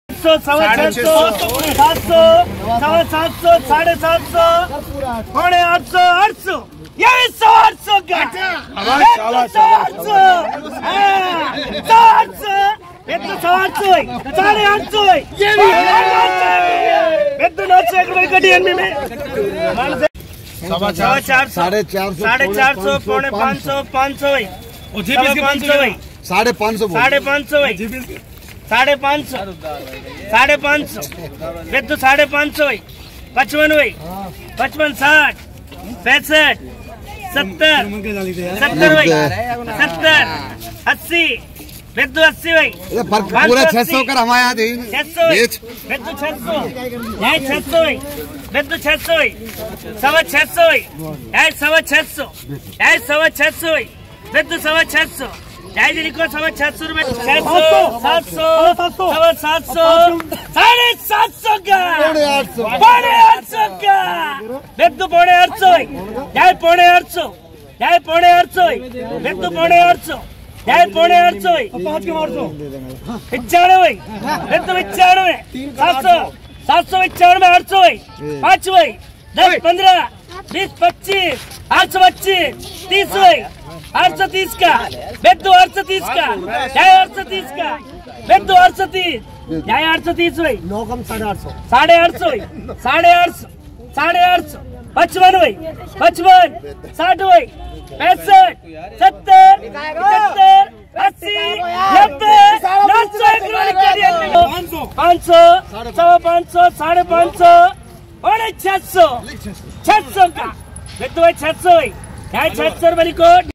अलवर मंडी प्याज की लाइव बोलीं प्याज भाव में रही तेजी 1/11/22
अलवर मंडी में प्याज की लाइव बोली लगती है और यह प्याज की बोलीं दिखाई जा रही है आज अभी तक प्याज का भाव 900 mon रहा